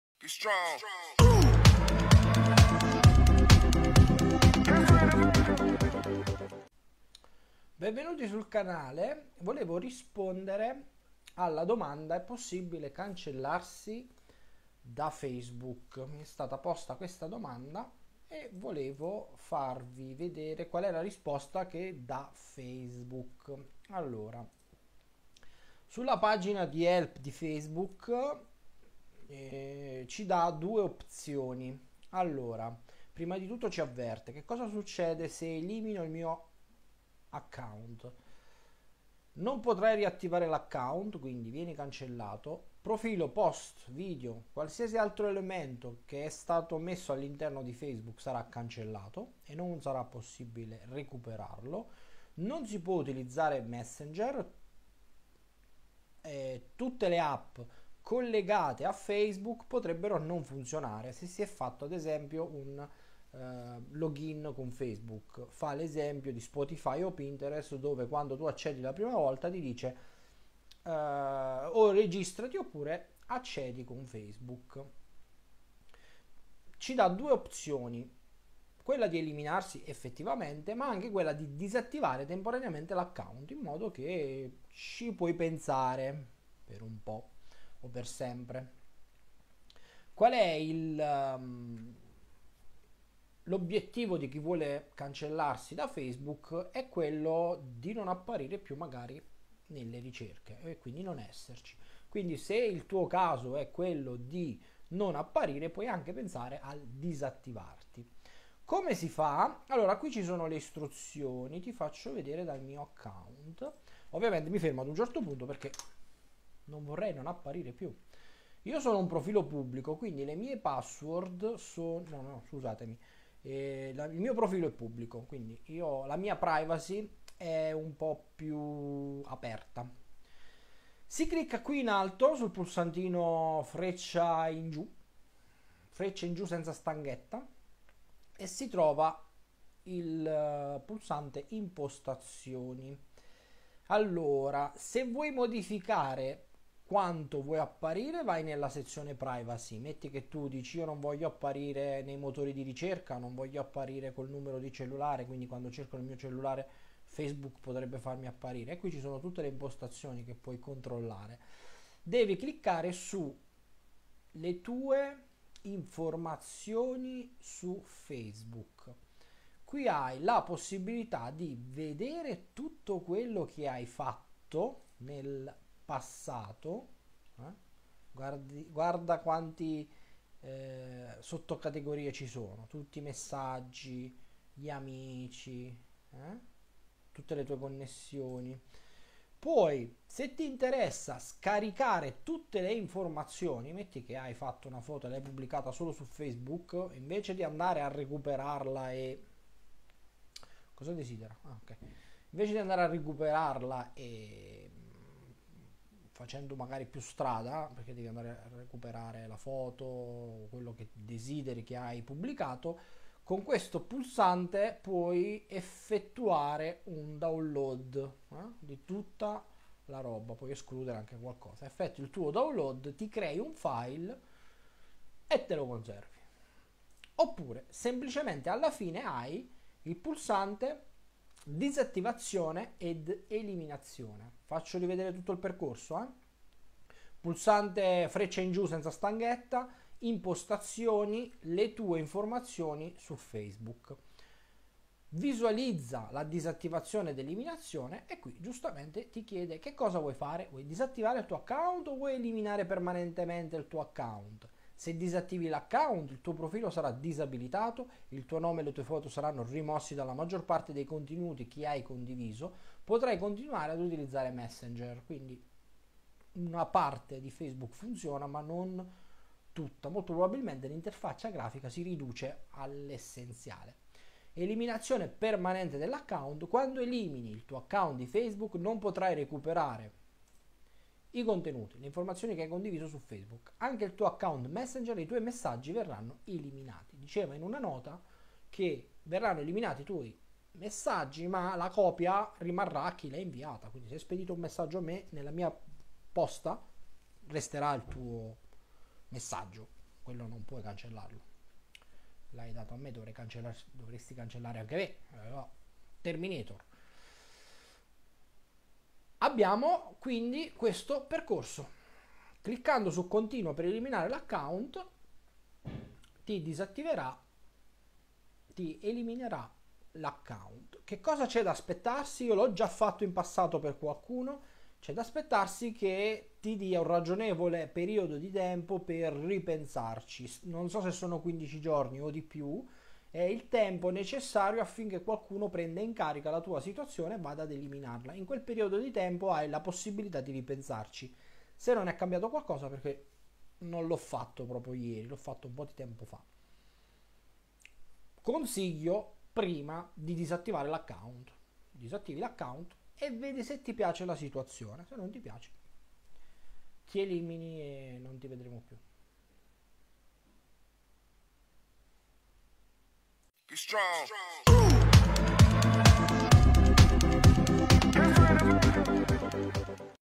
HelpDesk#02: E' possibile cancellarsi da Facebook? Sì!
Vuoi eliminare tutte le tue informazioni da Facebook? o per qualsiasi altro TUO volere ecco un video che ti spiega come fare.
Ti conisglio di disattivarti per un periodo e vedere se questo basta, ma nulla ti vieta di scomparire da questo sistema, per sempre o fino ad una nuova registrazione.
Grazie di avermi posto questo quesito!

Mi trovi su Facebook per info, chat ed video settimanali e HelpDesk